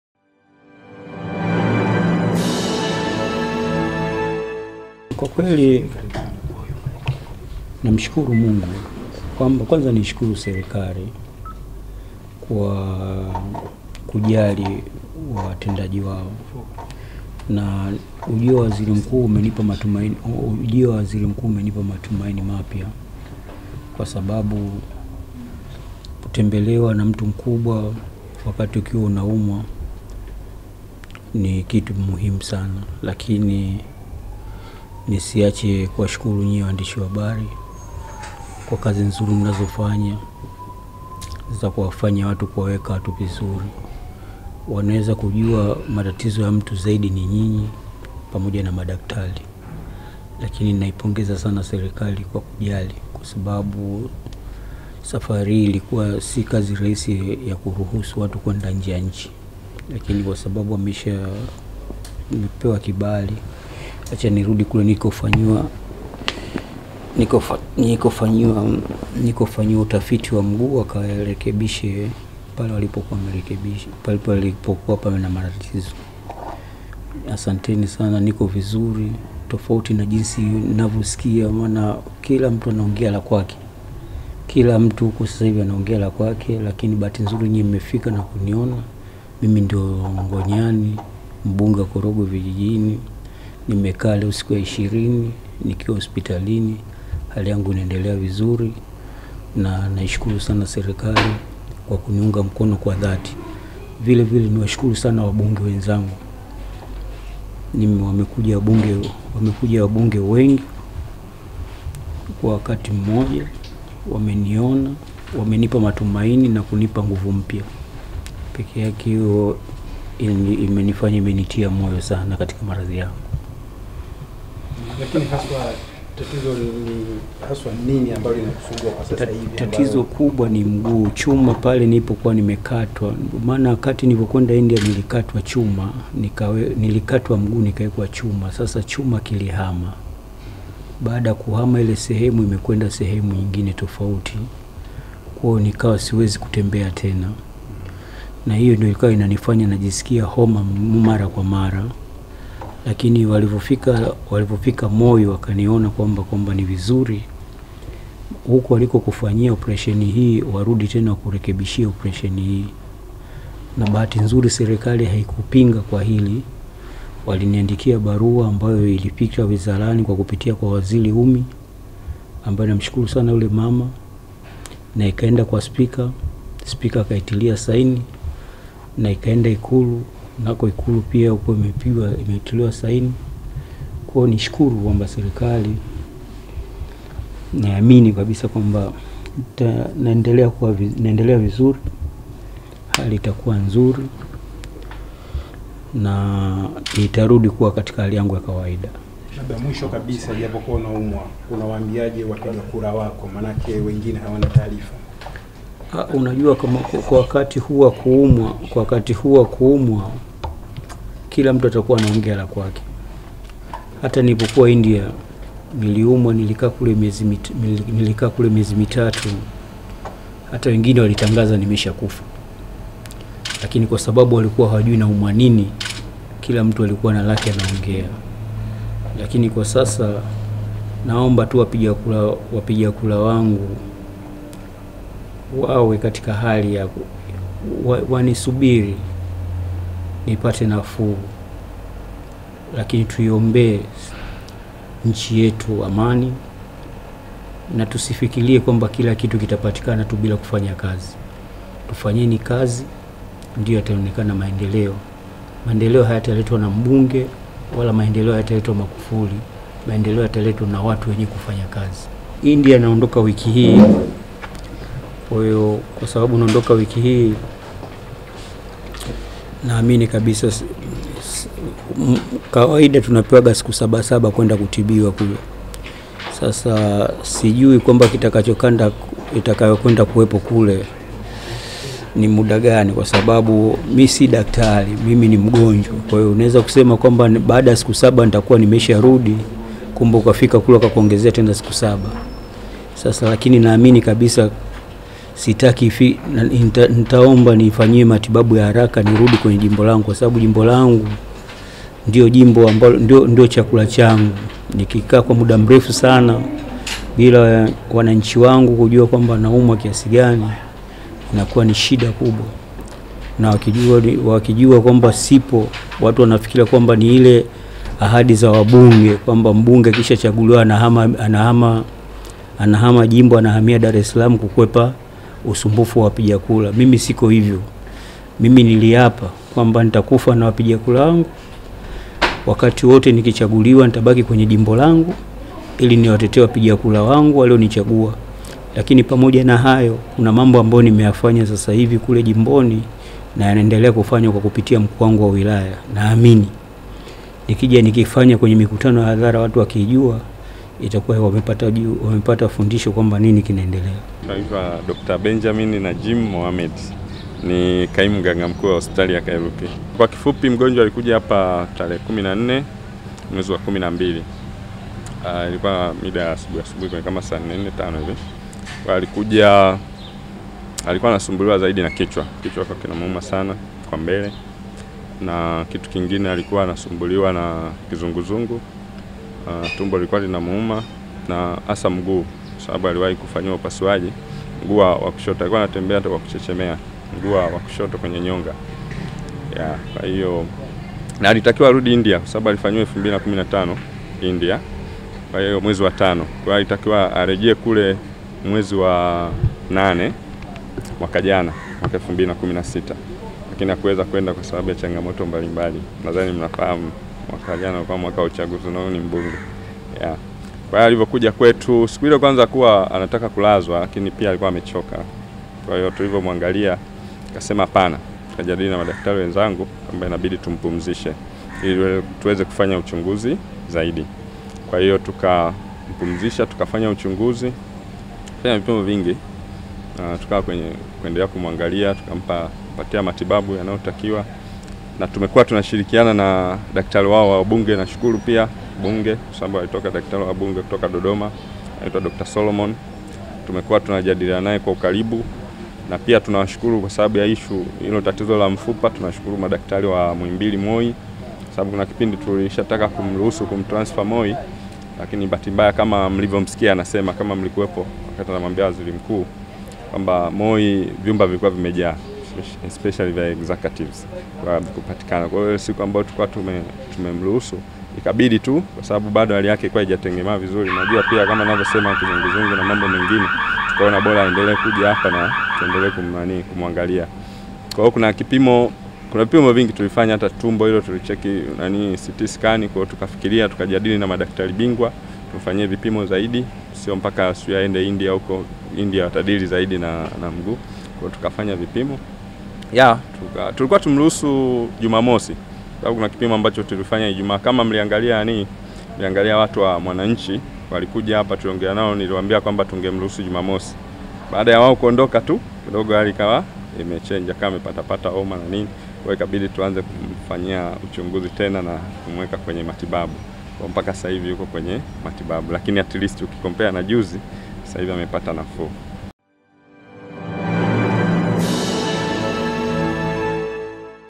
Prof. Maji marefu baada ya kutembelewa na waziri Mkuu hospitalini
Mbunge Steven Ngonyani Maji Marefu aliyelazwa katika Hospitali ya Kairuki DSM, anasumbuliwa na mfupa wa mguu uliowekewa chuma ambacho kimehama.

Prof. Maji Marefu anatarajia kwenda India kwa matibabu kutokana na tatizo la mguu unaomsumbua